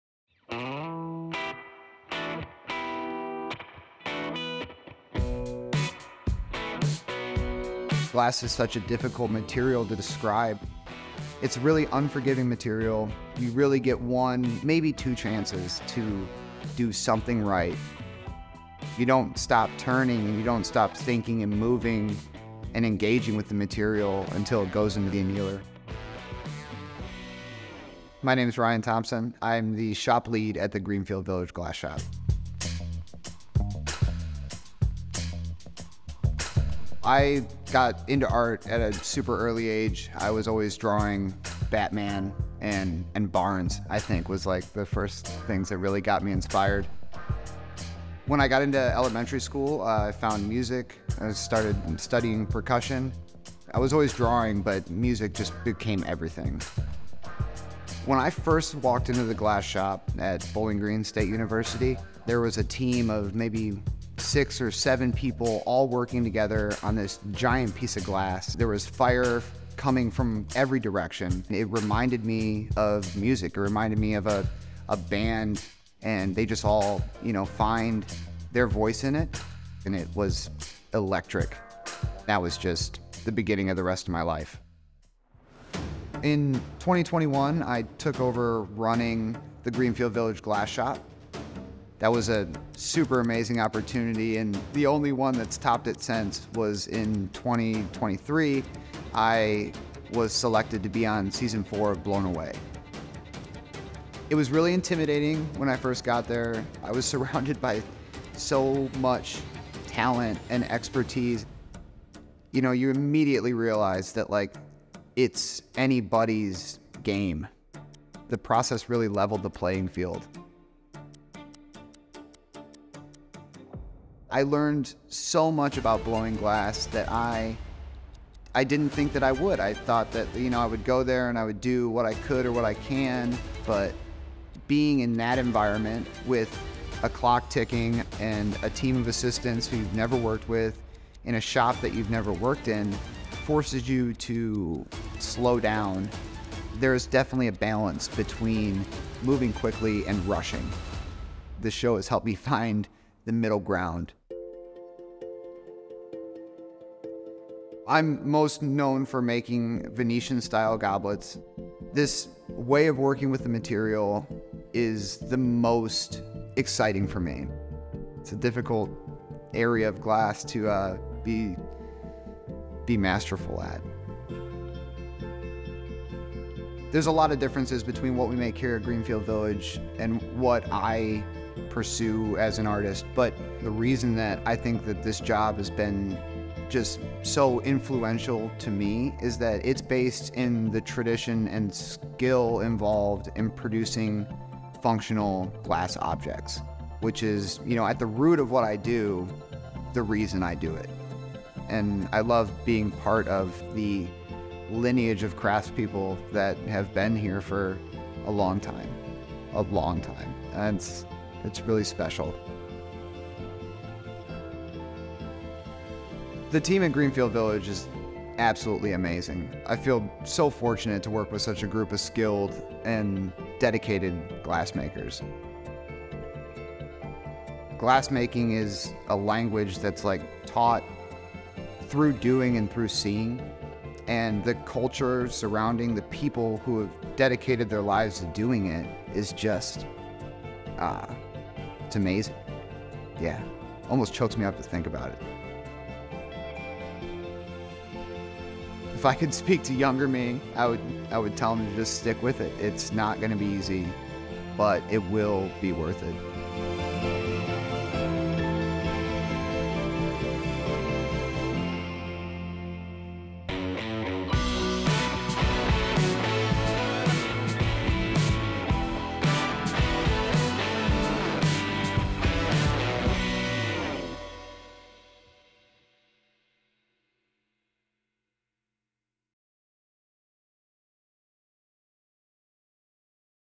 Meet Glass Blowing Artist Ryan Thompson
Meet Ryan Thompson, Greenfield Village Glass Shop lead and competitor on season four of the Netflix series Blown Away.

Thompson was drawn to art and music at an early age, which led him to earn his Bachelor of Fine Arts degree from Bowling Green University. He often compares glassmaking to performing in a musical ensemble: “Much like music, glassblowing is a universal language. Its techniques transcend culture and unite its practitioners from around the globe. Together, a group of skilled and focused artisans can work in near perfect harmony to achieve the shared goal of creating something beautiful.”

You can see Thompson go up against nine other extraordinary artists on Blown Away, and you may be able to spot him working at the Glass Shop during your visit to Greenfield Village.